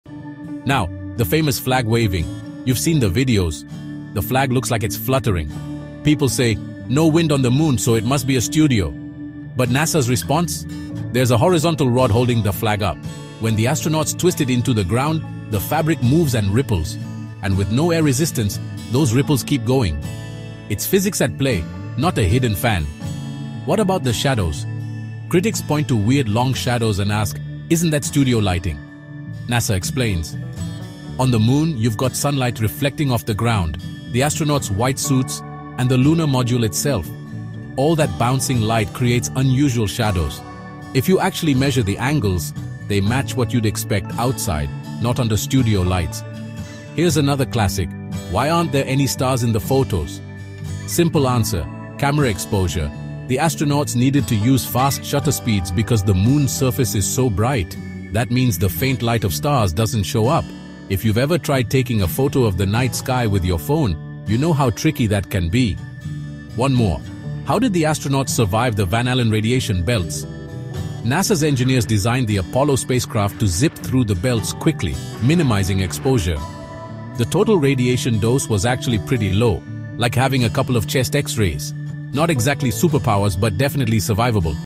Moon Landing Debunking the Famous Conspiracy Theories
In this video, we debunk the most famous moon landing conspiracy theories with clear evidence and scientific facts. Discover why the 1969 Apollo 11 mission was real and how myths and misconceptions were debunked once and for all!